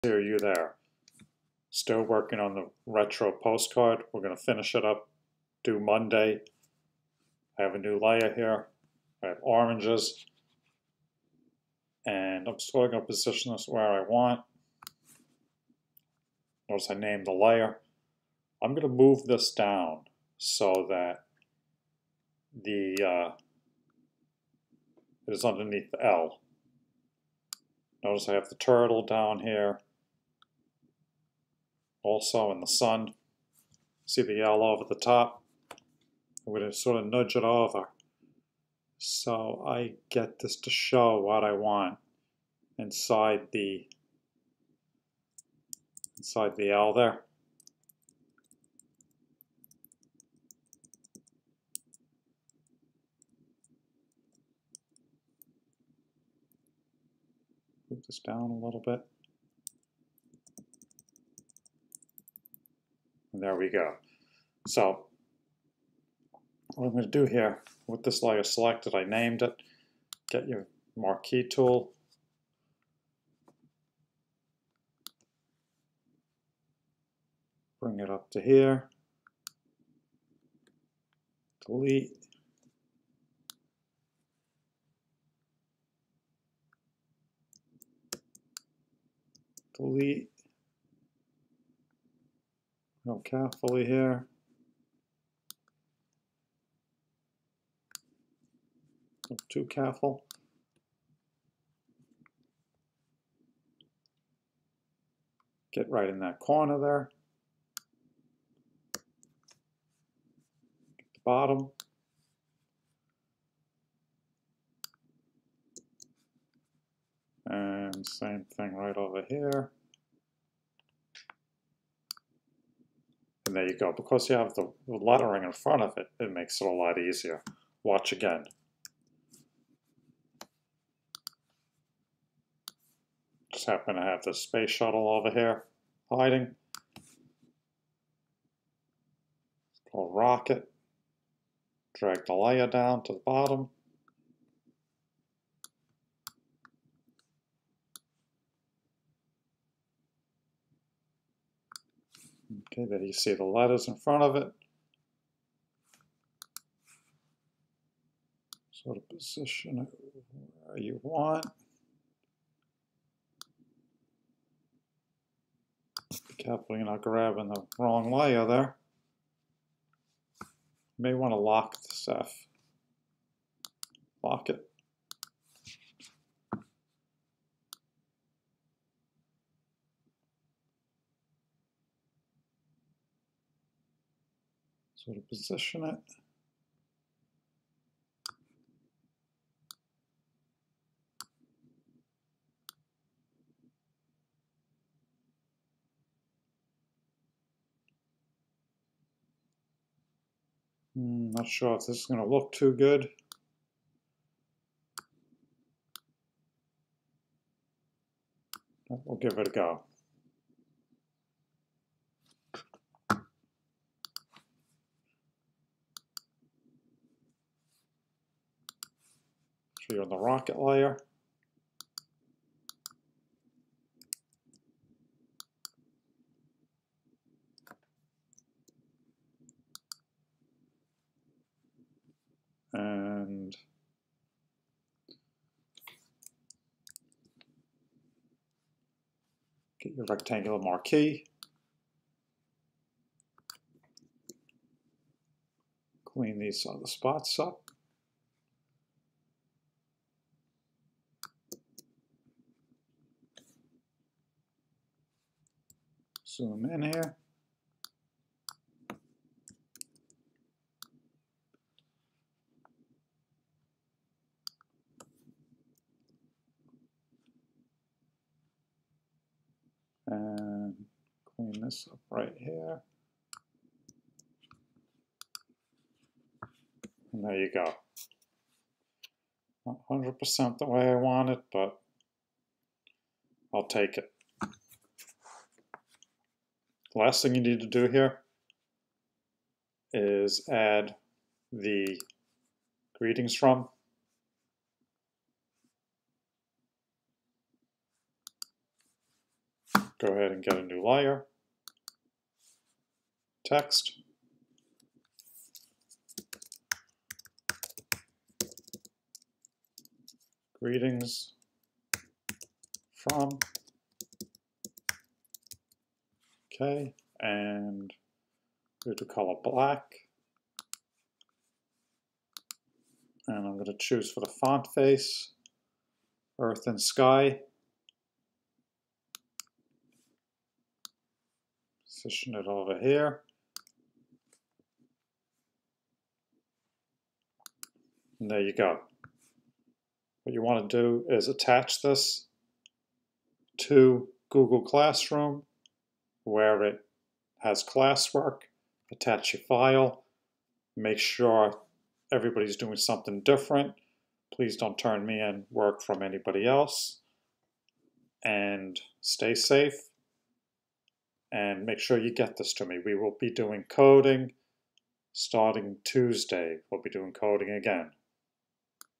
postcardPart5
Retro postcard using pixlr.com.  Tip on layers and mixing text with an image layer for the lettering.